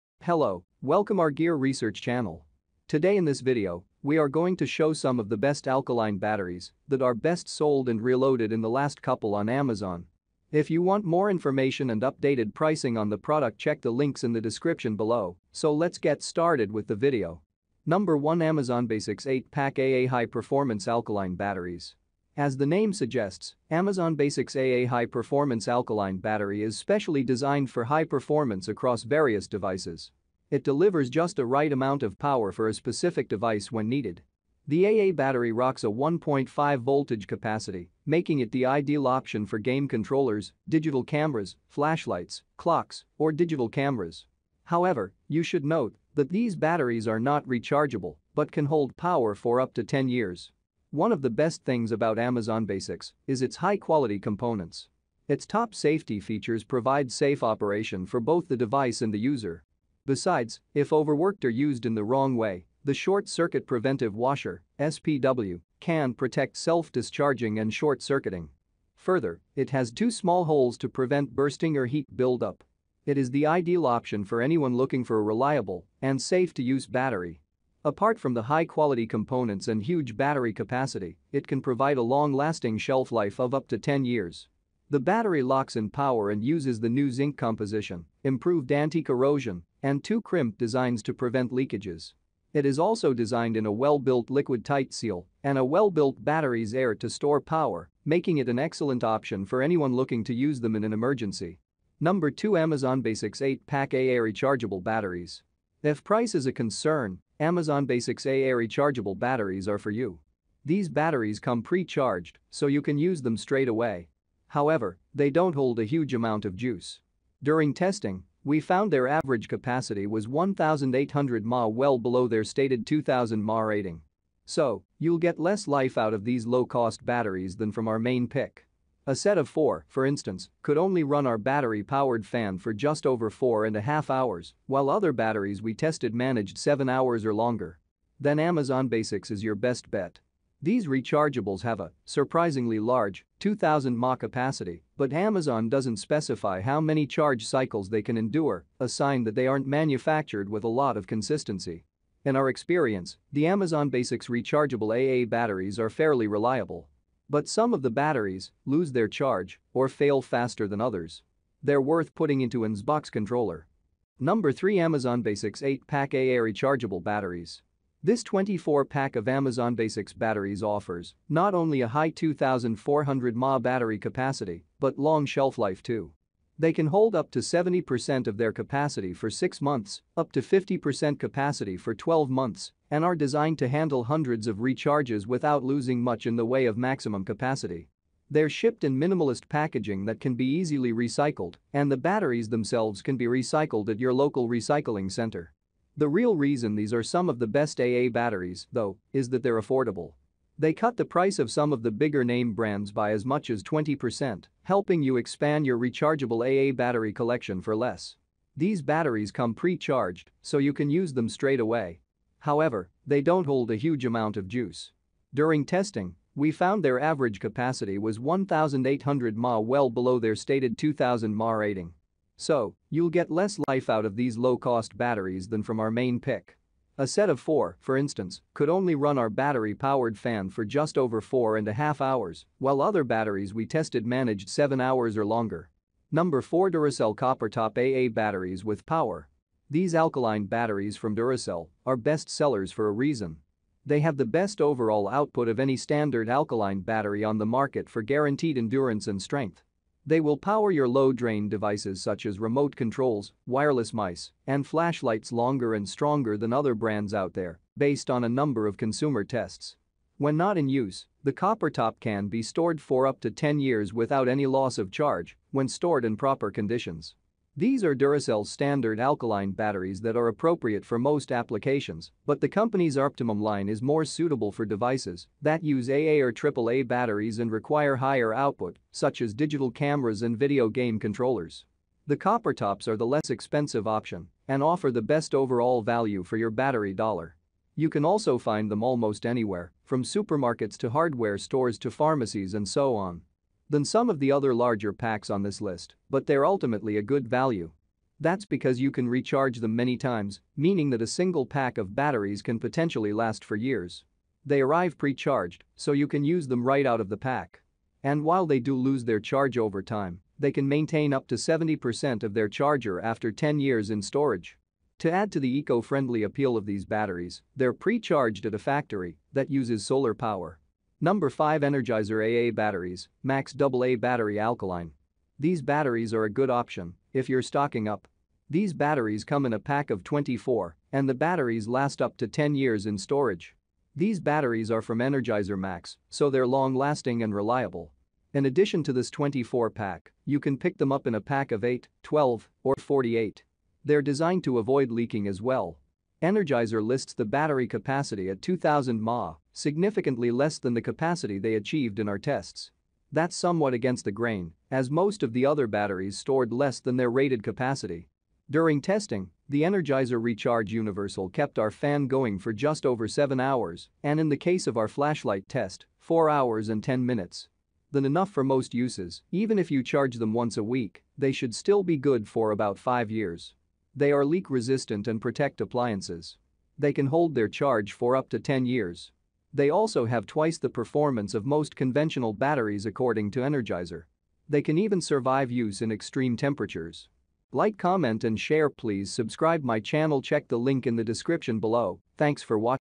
✅ Top 5: Best Alkaline Batteries [Tested & Reviewed]
📌Product Link📌:
☄️1 AmazonBasics 8 Pack AA High-Performance Alkaline Batteries.
📌Global Prices
☄️2 AmazonBasics 8-Pack AA Rechargeable Batteries.
📌Global Prices
☄️3 AmazonBasics 8-Pack AA Rechargeable Batteries.
📌Global Prices
☄️4 Duracell Coppertop AA Batteries with Power.
📌Global Prices
☄️5 Energizer AA Batteries, Max Double A Battery Alkaline.
📌Global Prices
☄️6 Duracell Coppertop AA Batteries with Power.
📌Global Prices
☄️7 Panasonic BK-3MCCA8BA eneloop AA 2100 Cycle.
📌Global Prices